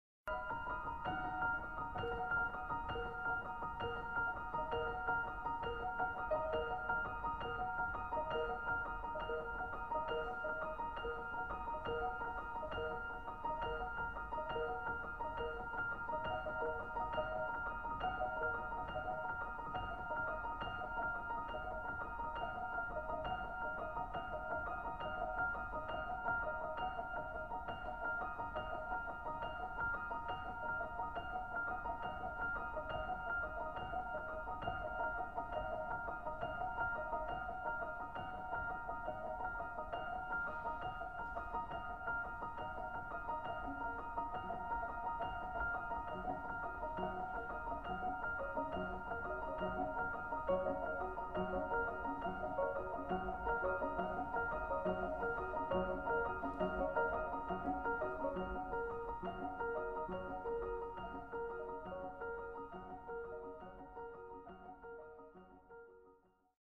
Rozenburg eggshell vase with poppies by Schellink 1903
A tall eggshell porcelain Rozenburg baluster vase, painted with red orange poppies. Model 90, 10¼ inch high, workorder number 111, year 1903, painters’mark Samuel Schellink